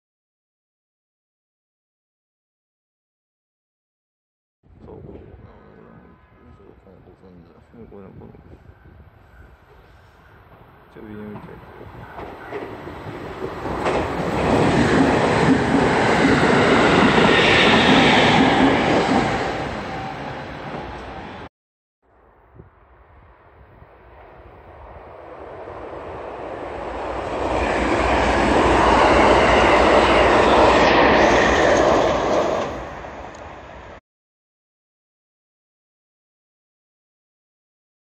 ７８７系ＢＭ１ 臨時特急かもめ６９号佐賀行 長崎本線神埼駅通過＆折り返し回送 伊賀屋～神埼にて
２０１８・１２・２８撮影
重要部検査明けで台車・パンタグラフがピカピカです